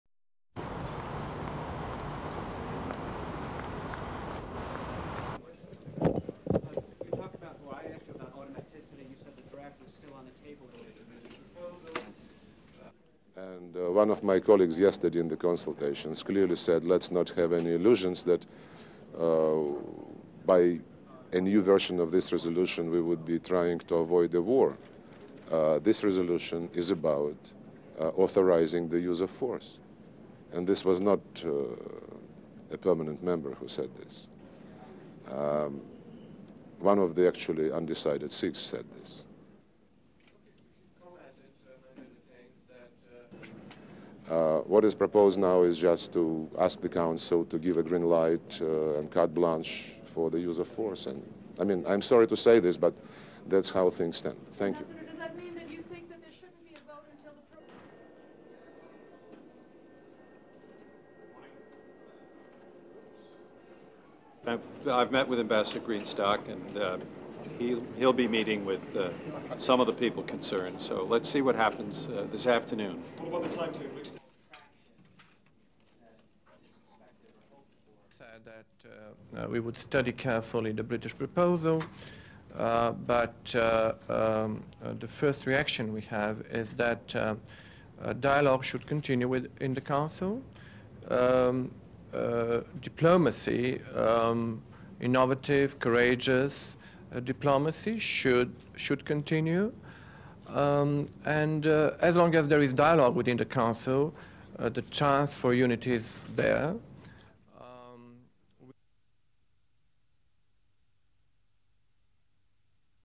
Various comments from UN ambassadors
APTN
1. Wide shot exterior UN
UNTV
2. Mediums shot of Russian Ambassador arriving
APTN
3. Cutaway of photographer
4. SOUNDBITE (English): Sergey Lavrov, Russian Ambassador to the U.N.:
"One of my colleagues yesterday in the consultations clearly said: let's not have any illusions that by a new version of this resolution we would be trying to avoid a war. This resolution is about authorising the use of force. And this was not a permanent member who said this, one of the actually undecided six said this."
5. Cutaway of press
6. SOUNDBITE (English): Sergey Lavrov, Russian Ambassador to the U.N.:
"What is proposed now is just to ask the council to give a green light, a carte blanche, for the use of force. And I mean, I'm sorry to say this, but that's how things stand."
7. Medium shot U.S. Ambassador waiting for elevator in U.N. building
8. SOUNDBITE (English): John Negroponte, U.S. Ambassador to the U.N.:
"I've met with Ambassador Greenstock (U.K.) and he'll be meeting with some of the people concerned, so let's see what happens this afternoon."
9. Wide shot of Bulgarian of Ambassador at microphone
UNTV
10. SOUNDBITE (English): Stefan Tavrov, Bulgarian Ambassador to the U.N.:
"We would study carefully the British proposal but the first reaction we have is that dialogue should continue in the council, diplomacy - innovative, courageous - diplomacy should continue, and as long as there is dialogue within the council, the chance for unity is there."
APTN
11. Wide shot of Bulgarian Ambassador at microphone
STORYLINE:
Russia's ambassador to the United Nations on Thursday described Britain's proposals on Iraq as "a green light, a carte blanche, for the use of force".
Sergey Lavrov added that one of the six non-permanent members, who has yet to decide its stance on the British plan, considered the latest version of the resolution to be about authorising the use of force.
Meanwhile, Bulgaria's ambassador to the UN,  Stefan Tavrov, said that his country's first reaction to the British draft was that diplomacy should continue.
U.S. Ambassador John Negroponte said he and his British counterpart Jeremy Greenstock were still holding meetings to try and resolve the impasse.
The fissure in the U.N. Security Council deepened on Thursday when France rejected a British compromise on Iraq, infuriating London and prompting the United States to consider postponing a vote on an ultimatum against Baghdad until next week.
Iraq, reveling in the turmoil at the council, dismissed Britain's plan, which lists six disarmament requirements Baghdad would have to meet or else face "serious consequences".
Great Britain proposed the list in a bid to win votes on the council for a U.S.-backed resolution authorizing war unless Baghdad meets a deadline.
To sweeten the offer, British officials also suggested pushing back the deadline from Monday, as originally proposed by the United States.
The United States had been hoping for a vote this week, but the White House announced on Thursday that President Bush was willing to put it off until next week if that would help gain support.
U.N. Secretary-General Kofi Annan on Thursday raised the possibility of a summit of a group of interested world leaders who are "searching for a compromise to get us out of this crisis".
He said such a summit, with leaders not necessarily on the Security Council, was suggested by Brazil.
France and Germany rejected the British proposals because they would still set a deadline for war.
The Security Council scheduled another meeting for Thursday afternoon to discuss the proposal.